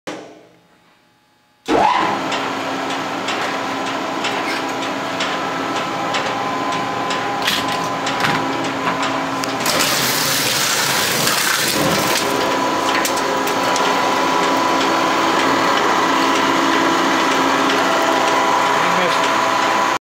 10hp shredder 16-150C Allegheny, Circuit board shred
Testing Allegheny 16-150C shredder, circuit board shred